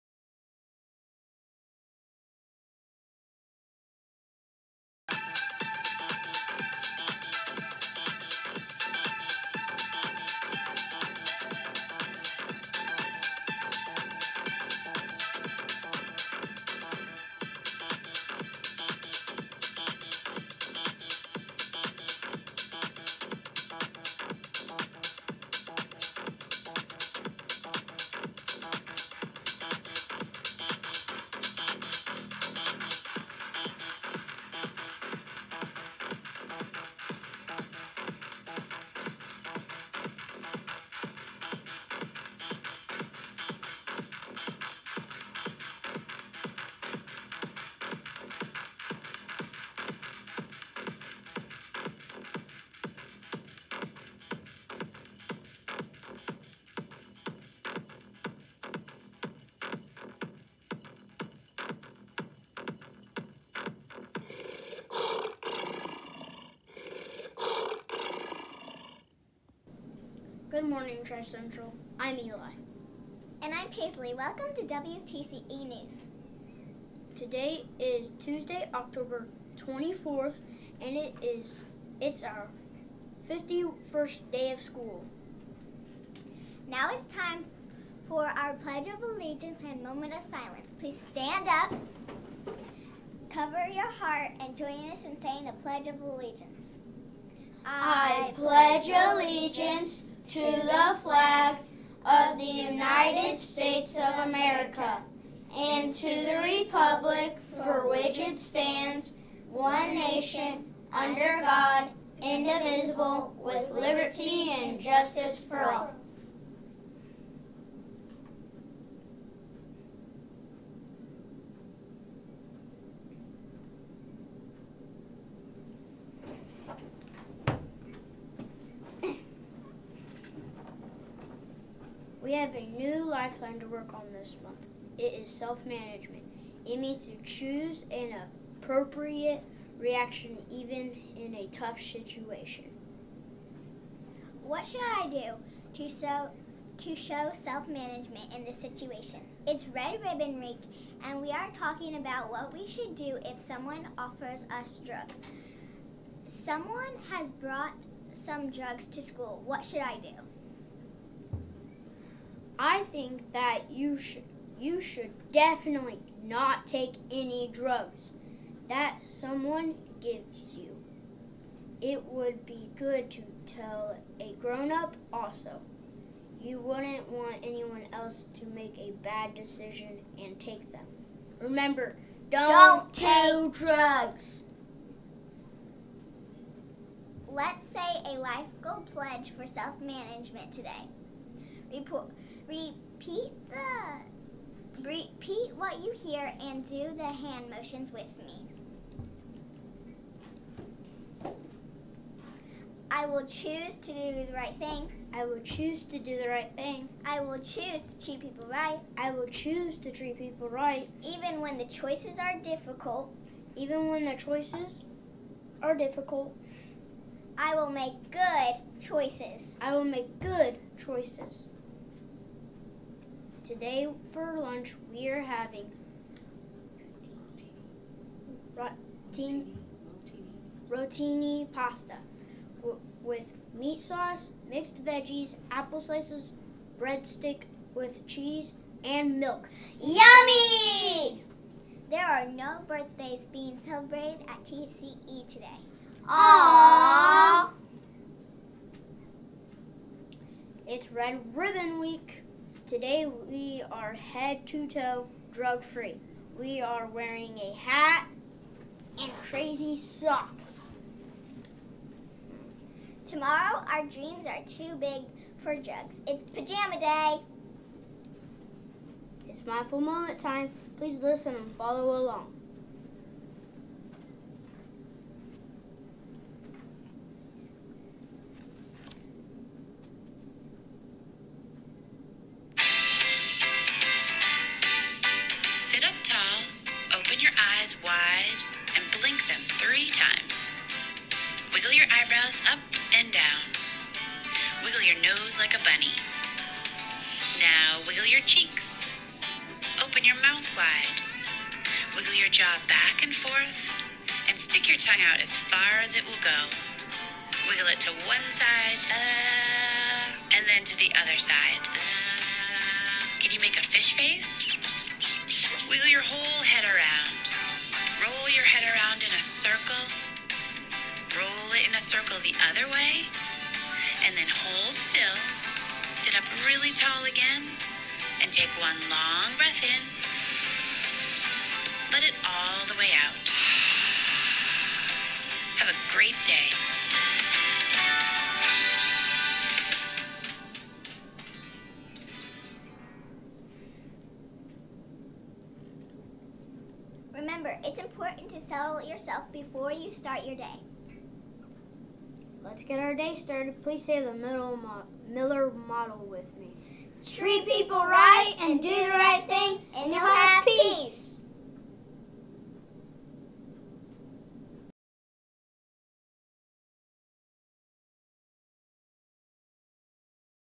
WTCE - Morning Announcements - Tuesday, October 24, 2023
It’s Red Ribbon Week! - Today is hat and crazy sock day! Fourth graders, Eli and Paisley are sporting some pretty fancy hats! Tomorrow is PJ Day!